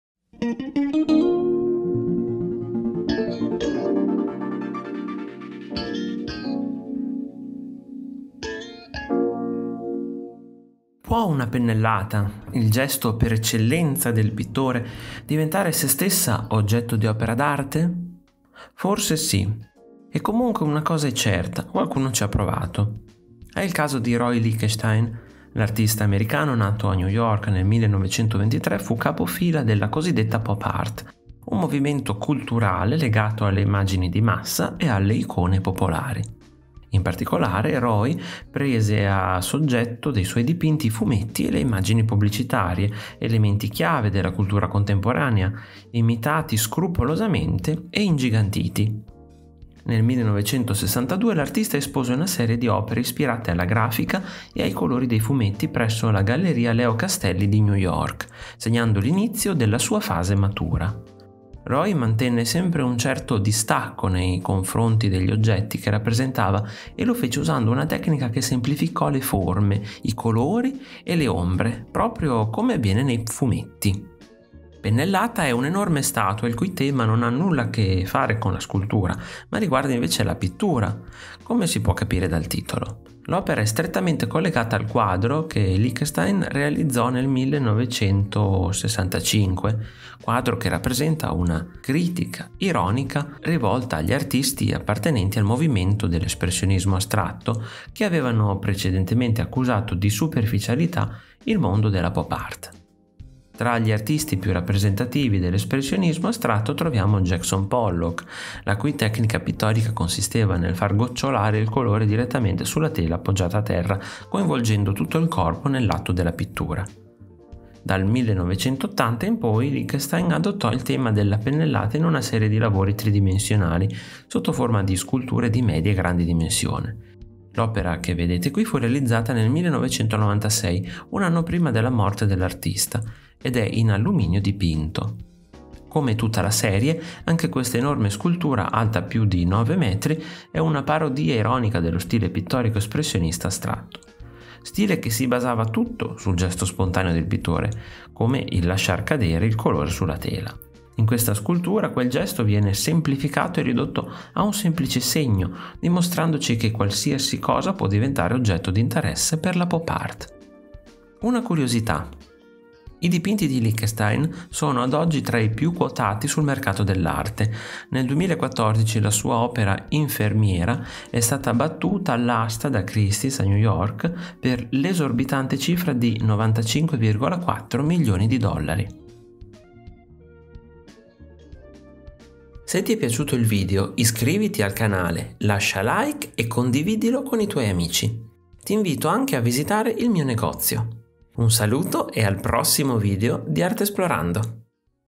Roy Lichtenstein - pennellata | storia dell'arte in pillole
La storia dell'arte in pillole è un nuovo viaggio di Artesplorando. Qui vi parlerò di pennellata di Roy Lichtenstein ▼▼CONTINUA SOTTO▼▼

Può una pennellata, il gesto per eccellenza del pittore, diventare essa stessa oggetto di un’opera d’arte? Forse sì, e comunque una cosa è certa, qualcuno ci ha provato. È il caso di Roy Lichtenstein. L’artista americano nato a New York nel 1923 fu capofila della cosiddetta “pop art”, un movimento culturale legato alle immagini di massa e alle icone popolari. In particolare Roy prese a soggetto dei suoi dipinti i fumetti e le immagini pubblicitarie, elementi chiave della cultura contemporanea, imitati scrupolosamente e ingigantiti. Nel 1962 l’artista espose una serie di opere ispirate alla grafica e ai colori dei fumetti presso la Galleria Leo Castelli di New York, segnando l’inizio della sua fase matura. Roy mantenne sempre un certo distacco nei confronti degli oggetti che rappresentava e lo fece usando una tecnica che semplificò le forme, i colori e le ombre, proprio come avviene nei fumetti.
Pennellata è un’enorme statua il cui tema non ha nulla a che fare con la scultura ma riguarda invece la pittura, come si può capire dal titolo. L’opera è strettamente collegata al quadro che Lichtenstein realizzò nel 1965. Quadro che rappresentava una critica ironica rivolta agli artisti appartenenti al movimento dell’espressionismo astratto che avevano precedentemente accusato di superficialità il mondo della pop art. Tra gli artisti più rappresentativi dell’espressionismo astratto troviamo Jackson Pollock, la cui tecnica pittorica consisteva nel far gocciolare il colore direttamente sulla tela appoggiata a terra, coinvolgendo tutto il corpo nell’atto della pittura.

LETTURE CONSIGLIATE
➡ Roy Lichtenstein. Opera prima. Ediz. illustrata